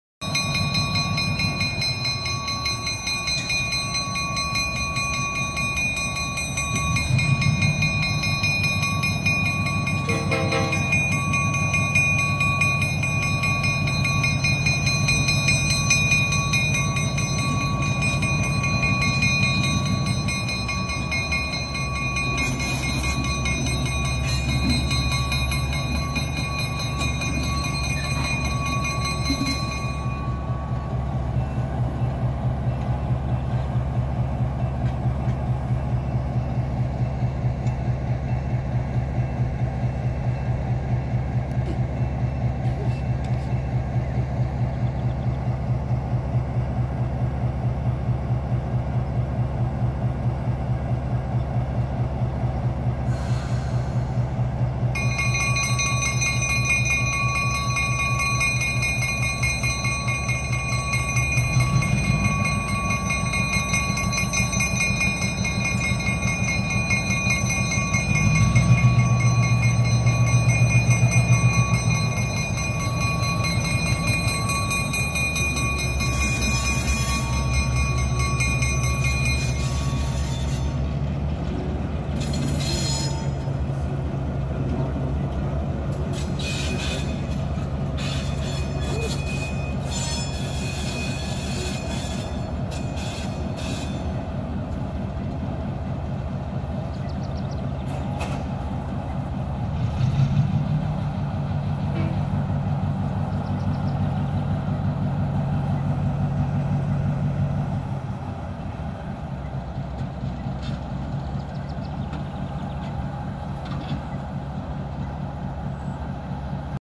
Former I&M RS1325 (C&IM) #30 works the IRM locomotive yard 7/8/23
First time seeing former C&IM 30 in person and it did not disappoint! Caught working the locomotive yard at IRM